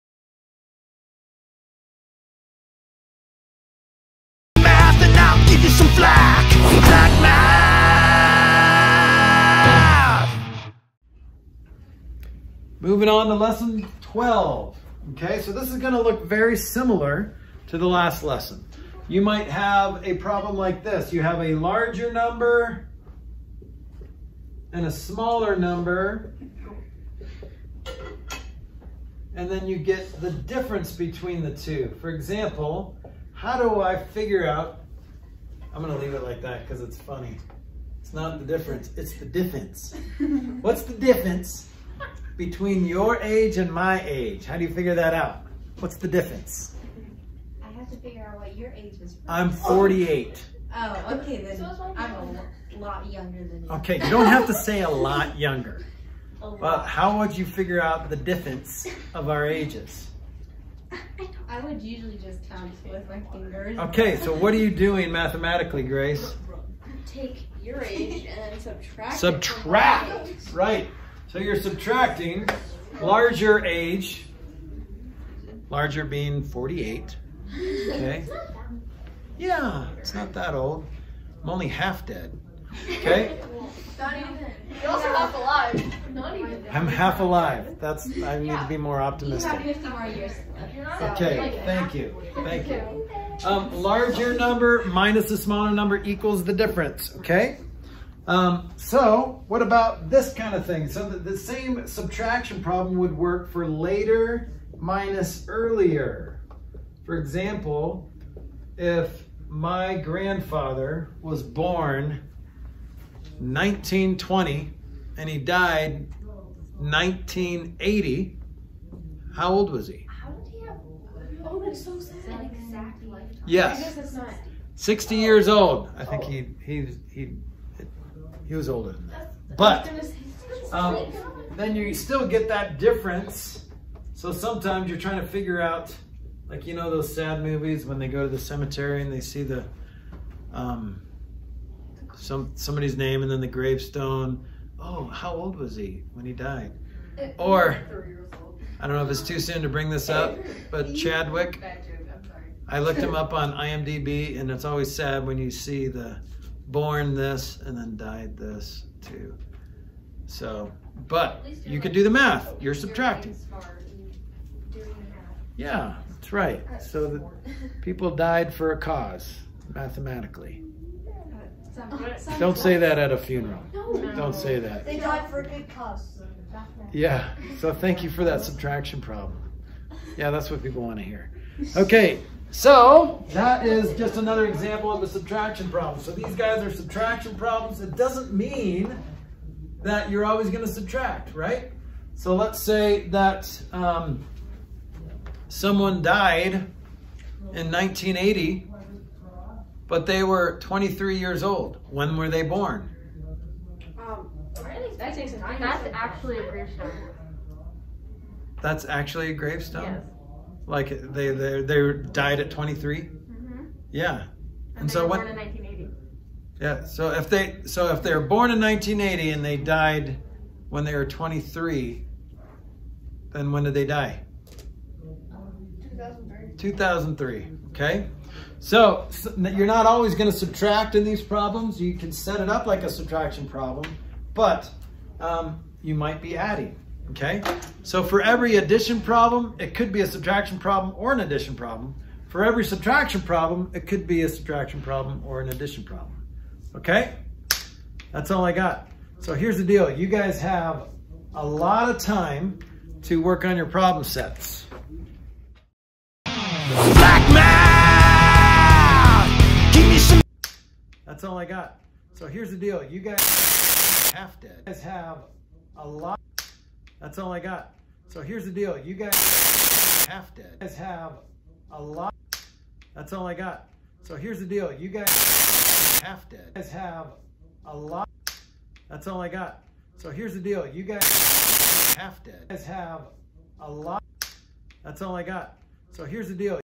Saxon Math 87 Lesson 12 - Problems About Comparing
Saxon Math 87 Lesson 12
Objective: Problems About Comparing, Problems About Separating